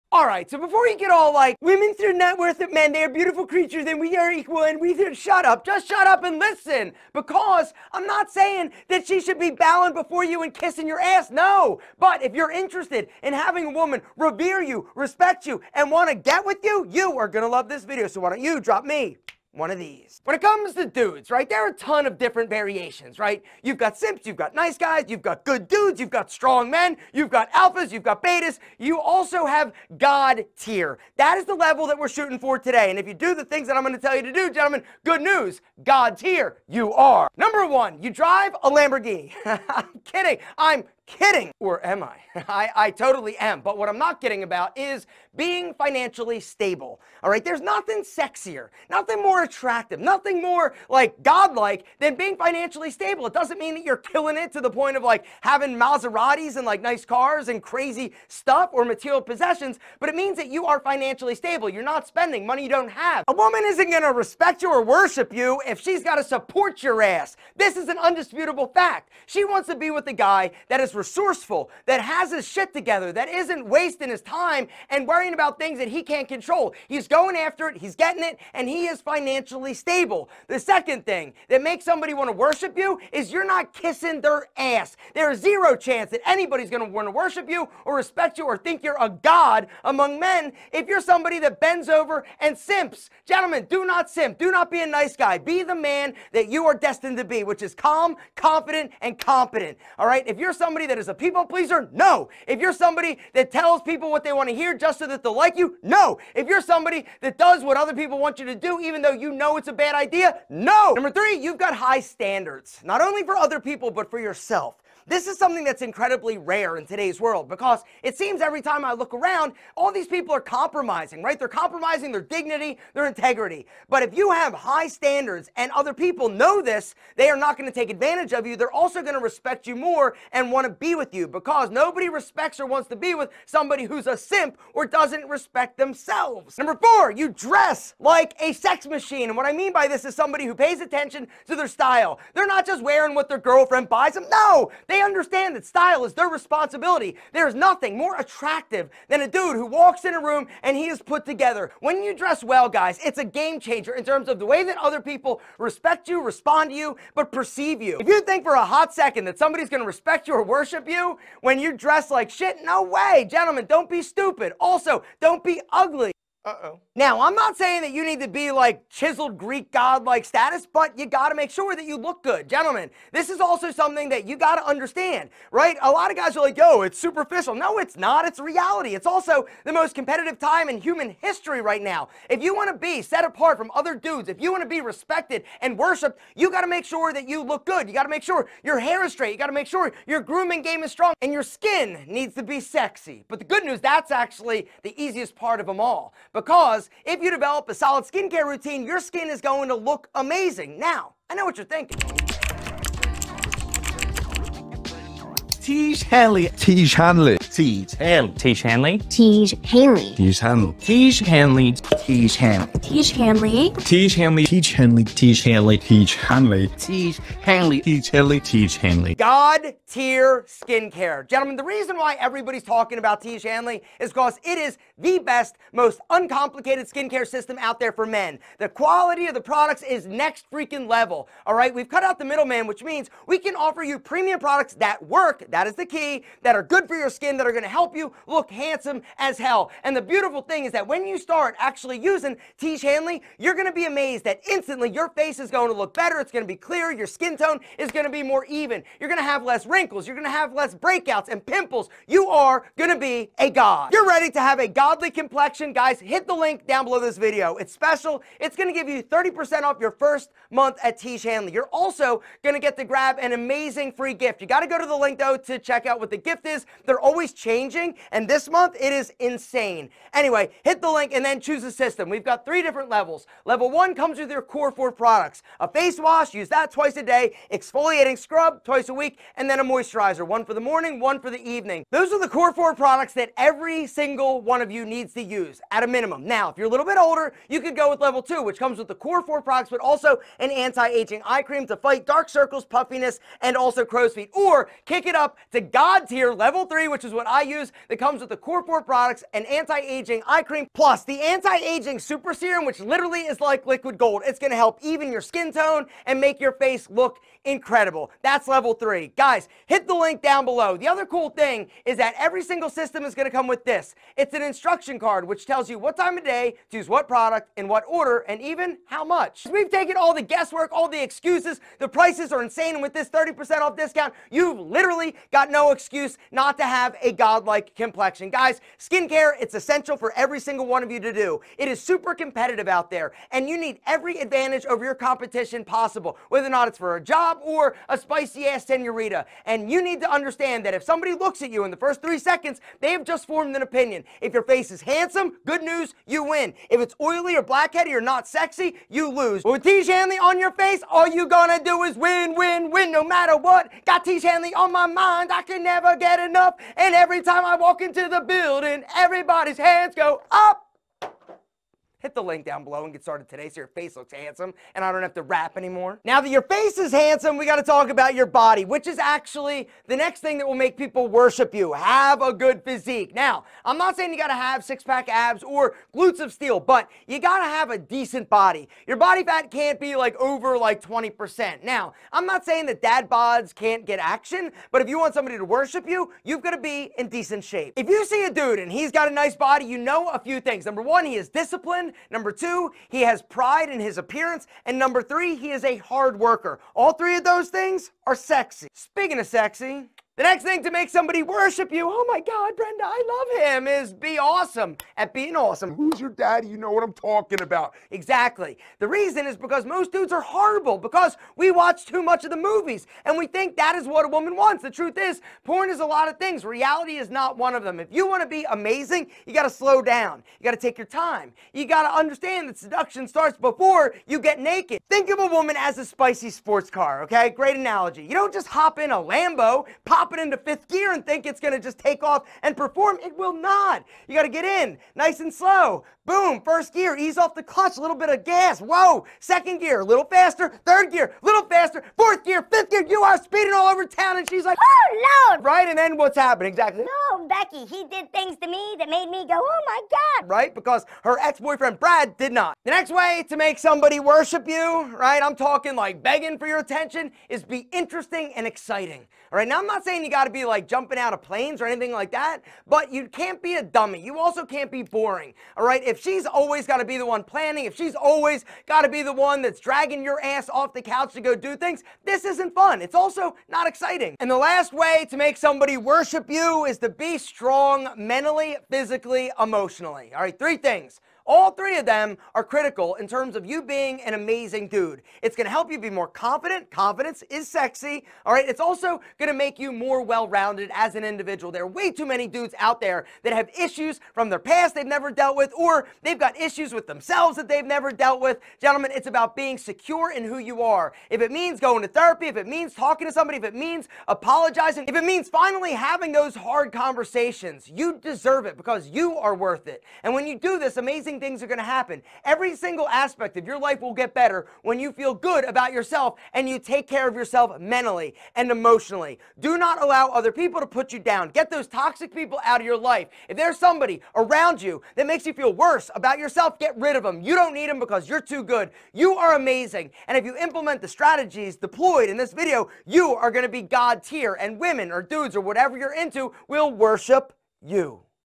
How to Make Her WORSHIP You! (6 Psychological TRICKS)
You will love this video if you want to have a woman revere, respect, and get with you. Dudes come in a wide variety: simps, nice guys, good dudes, strong men, alphas, betas, and god tier. That top tier, the god tier, is the level we're shooting for today. If you do the things I will relay, good news, she will worship you. She will revere you if you ...

Have financial stability. There's nothing sexier and more attractive than being financially stable. This doesn't mean that you're killing it to the point of having Maseratis, but it means that you are financially sound and not spending money you don't have. Women will not worship you if they have to support your ass. They want to be with a resourceful guy that has his shit together.

Are not an ass-kisser. There's zero chance she will worship or respect you if you bend over and simp. Do not be a nice guy or a people-pleaser telling people what they want to hear. Be the man you are destined to be, calm and confident. High standards not just for yourself but for other people. It's very rare to see people not compromise their dignity in today's world. If you have high standards, and others know this, they won't take advantage of you. They will respect you more and want to be with you because nobody respects someone who doesn't respect themselves.

Dress like a sex machine. Pay attention to your style and realize it's your responsibility. There's nothing more attractive than a dude who walks into a room put together and dressed well. It's a game-changer. Ensure you look good and set yourself apart from other dudes. Ensure your hair and grooming games are strong and your skin is sexy. Tiege Hanley has the best and most uncomplicated skincare system out there.

Have a good physique. Have a decent body with a body fat no higher than 20%. Dad bods can get action, but you must be in decent shape if you want to be worshiped. Be disciplined, have pride in your appearance, and be a hard worker.
Are seductive. Slow down and take your time! Understand the seduction starts before you get naked.

Are interesting. You don't have to jump out of airplanes, but you can't be a dummy. You can't be boring! Make some plans with her and get your ass off the couch.

Are strong. Be strong mentally, physically, and emotionally; these three are critical for being amazing. Confidence and being a well-rounded individual are sexy. Deal with your issues from your past, be secure and who you are, and have hard conversations if needed. Get toxic people out of your life too.